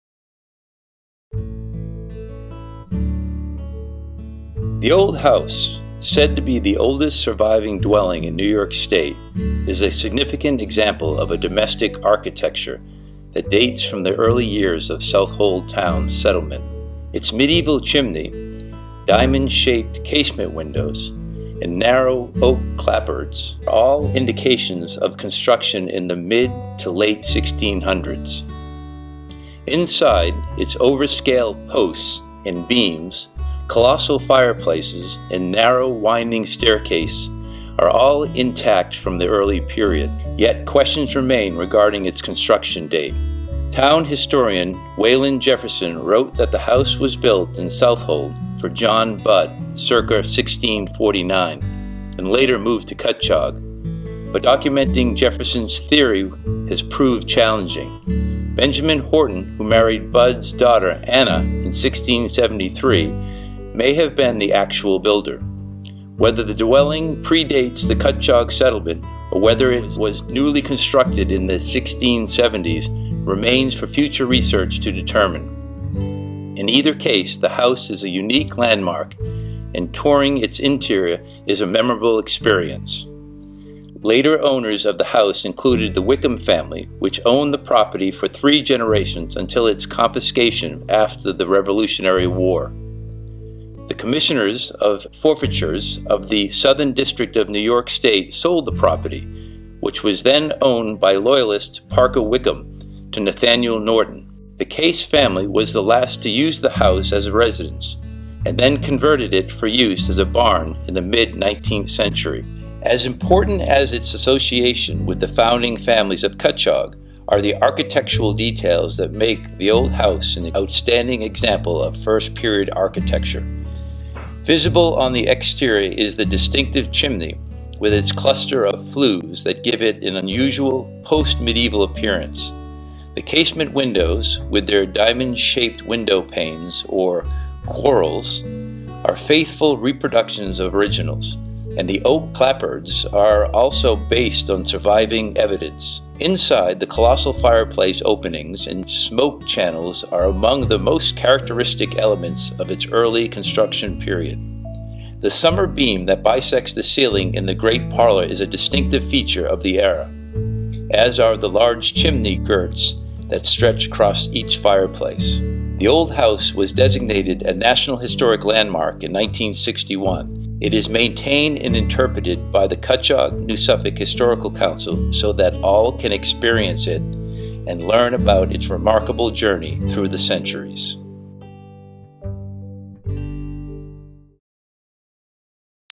The Old House - Cutchogue Village Green, Cutchogue, New York - Official Video from CNSHC
THE OLD HOUSE, built in 1649 in Southold, was moved to Cutchogue in 1660, It was restored in 1940 and designated a National Historic Landmark in 1962. The oldest English-type house in New York State, it is considered to be one of the superb examples of First Period architecture in the nation. Its design combines the finest architectural features of 17th century dwellings, such as sitting room paneling, leaded glass windows, and the massive fluted chimney. Visit the Old Schoolhouse Saturday., Sunday, and Monday during the summer months.